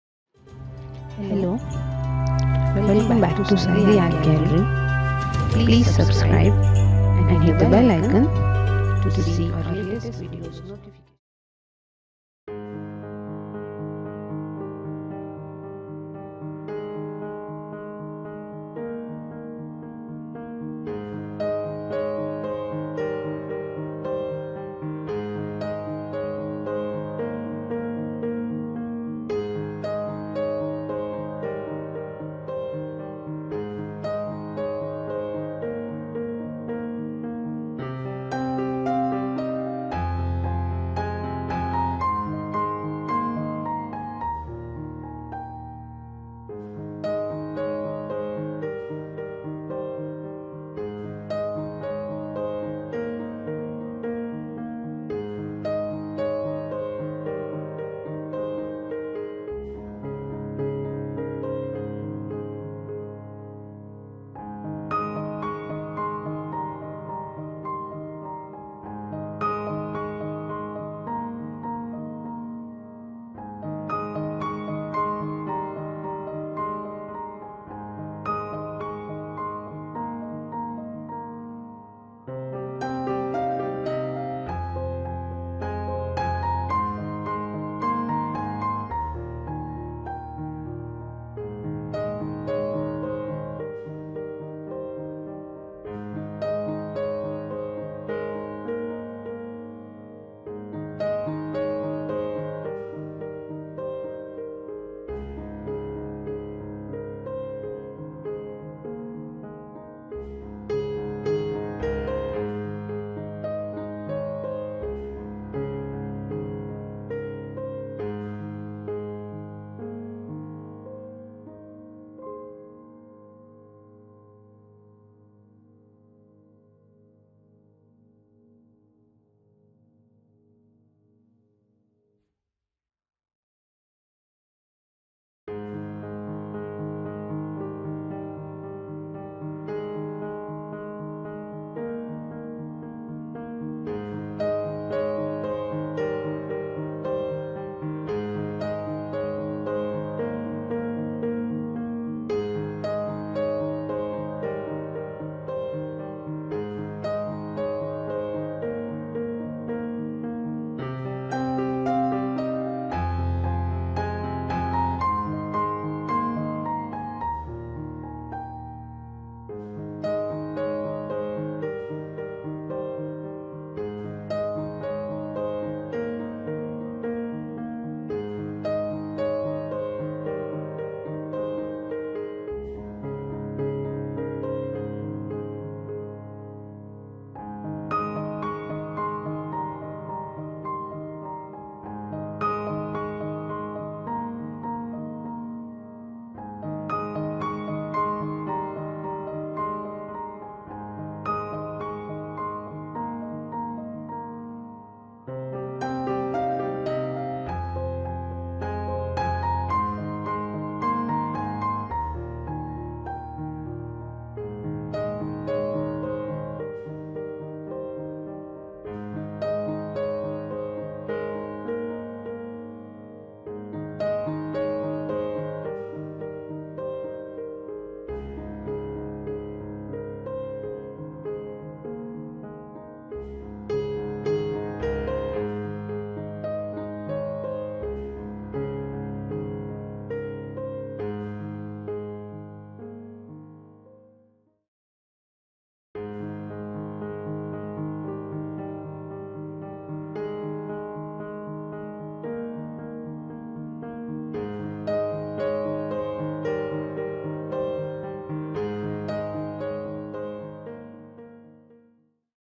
New peacock mehndi design for back hand
Hello friends,

In this video i am trying Beautiful peacock henna design on hand.

If you really like our efforts then please

XOXO
SAG